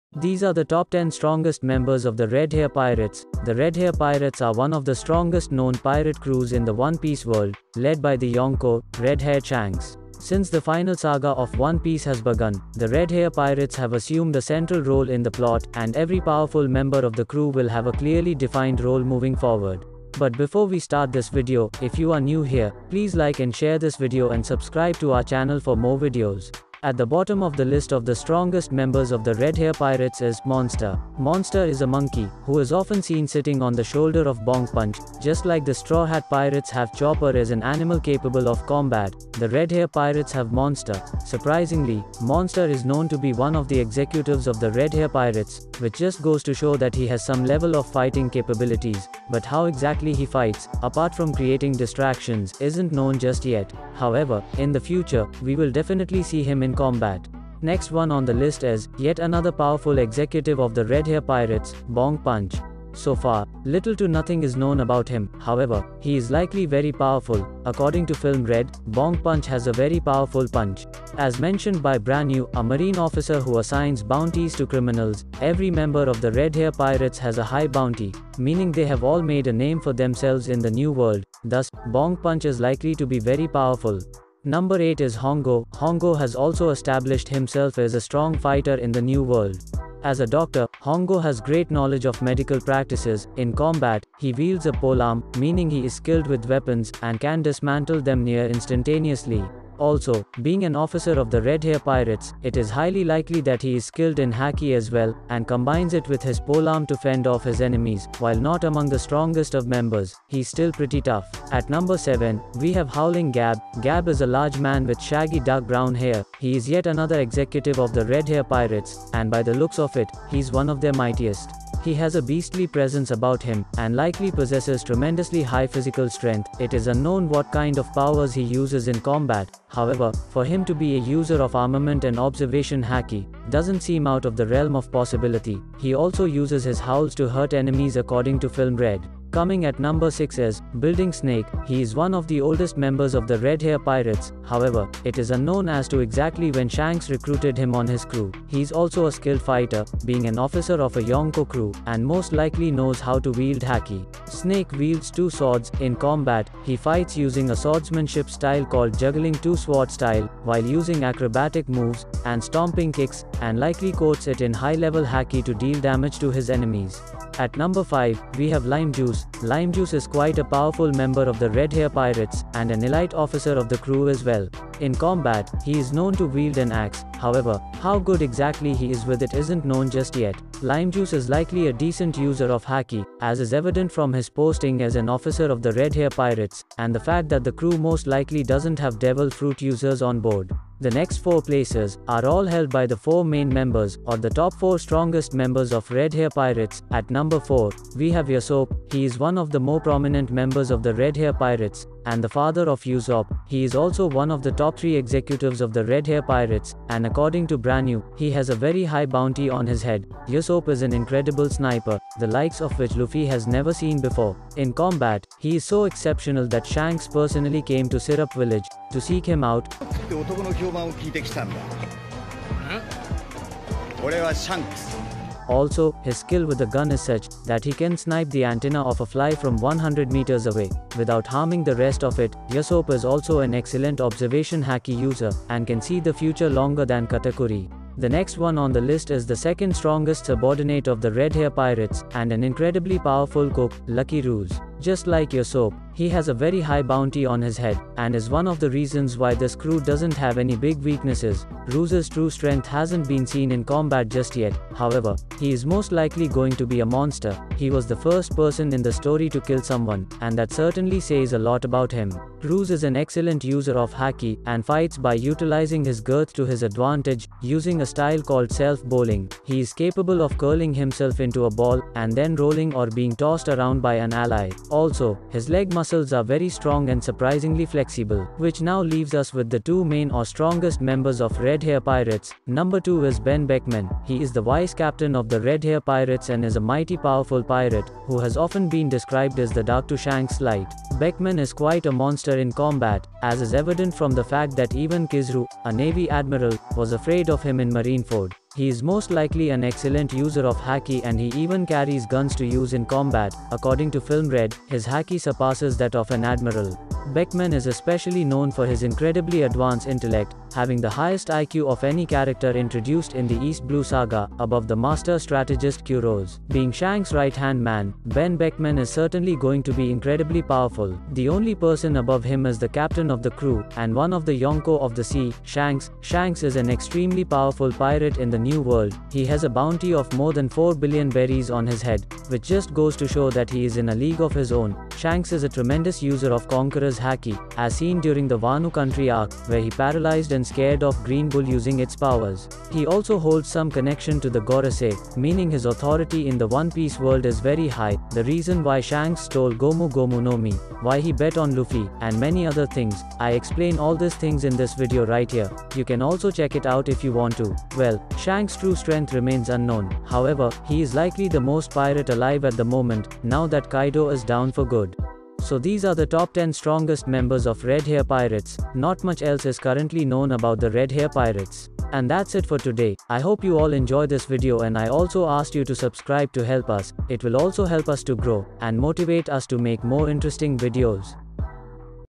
All Top 10 Strongest Members of The Red Haired Pirates | One Piece
Hey, Guys New Video Here. ,
- In This Video I'm Going to Talk About All Top 10 Strongest Members of The Red Haired Pirates in One Piece
- I Hope You Guys Will Like This Video
TITLE:- All Top 10 Strongest  Members of The Red Haired Pirates